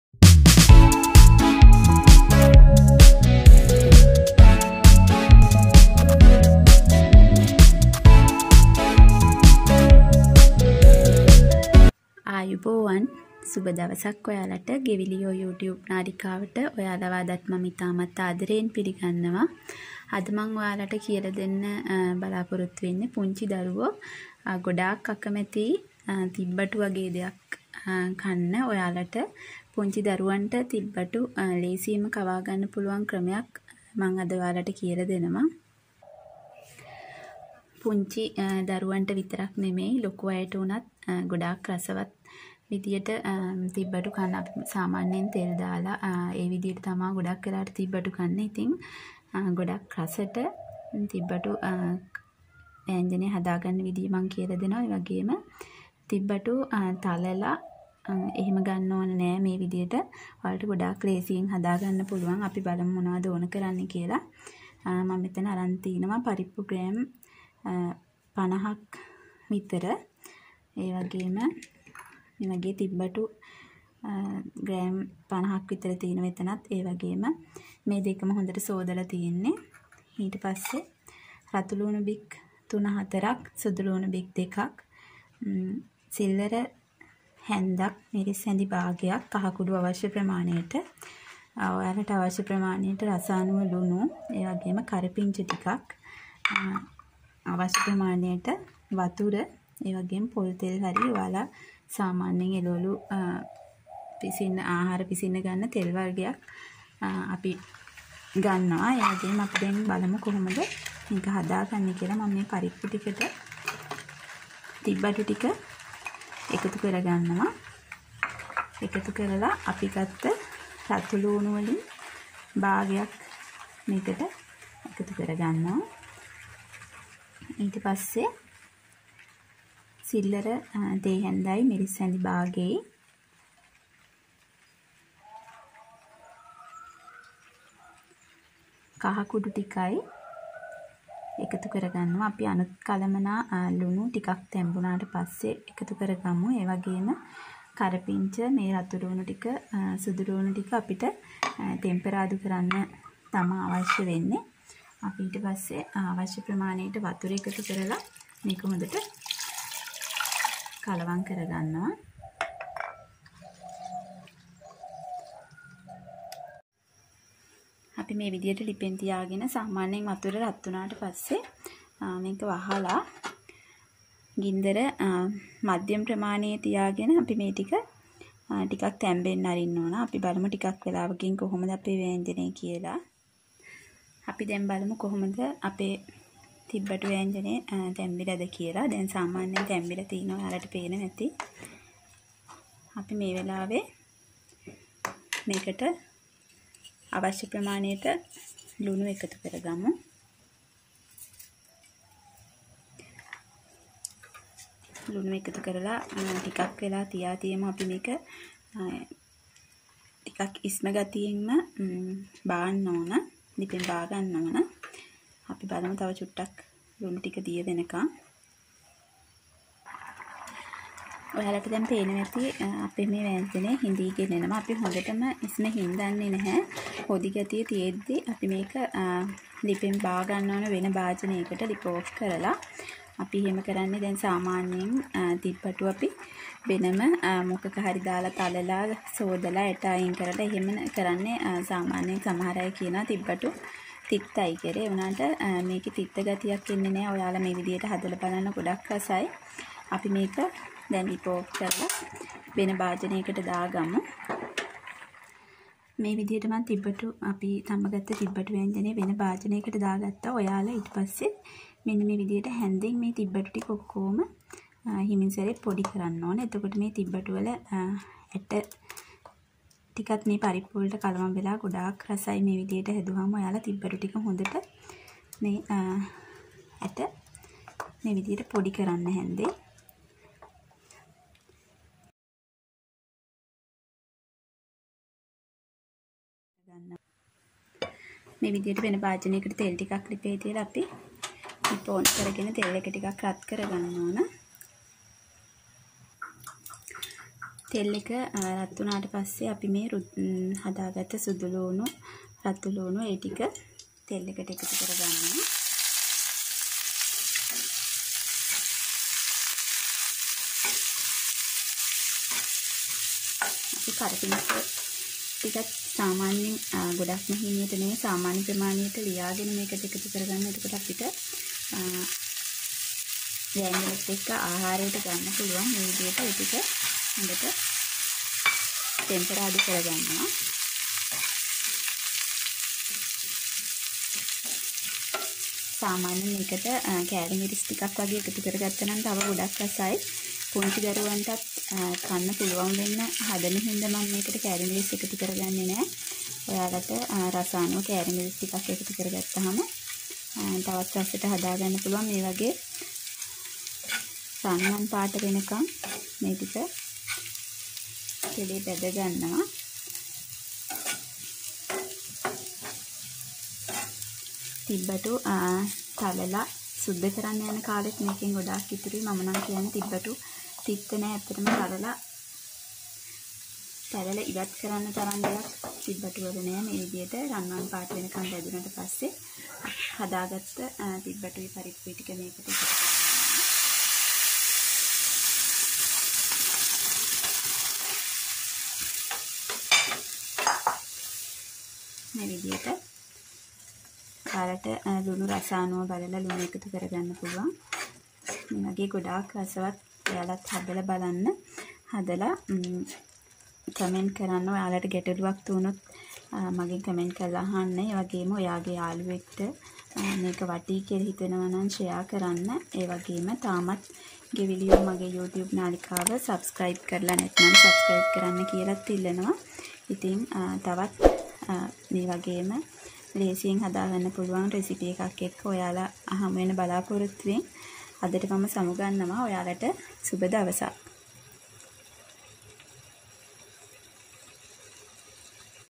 තිබ්බටු වෑංජනය /Thibbatu curry/Sl geviliyo /ගෙවිලියෝ/ 🇱🇰
thibbotu curry

Ingredients:
 lentils 50g
tibbatu 50g
salt powder,
half a tablespoon chilli,
a small spoon,
a little turmeric powder,
 5 onions,
two garlic cloves,
curry leaves
, a little oil,
 enough water